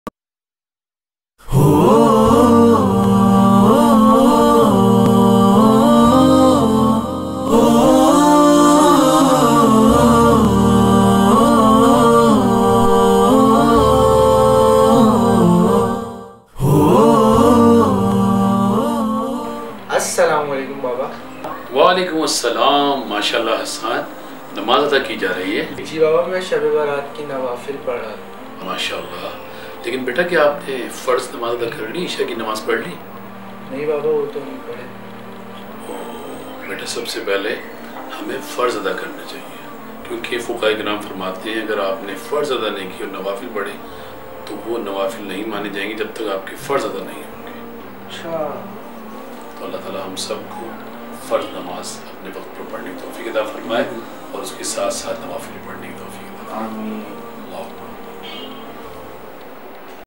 Beautiful mesage of Shab e Baraat | Imran Shaikh Attari & Hassan Shaikh Attari
Beautiful mesage of Shab e Baraat | Imran Shaikh Attari give a message to his son Hassan Shaikh Attari -imran shaikh attari officials

imran shaikh attari
imran shaikh attari officials
Beautifull message about Shabe Baraat:  is observed on the 15th night of the Arabic month of Shaban. Muslims offer special prayers, recite the Quran.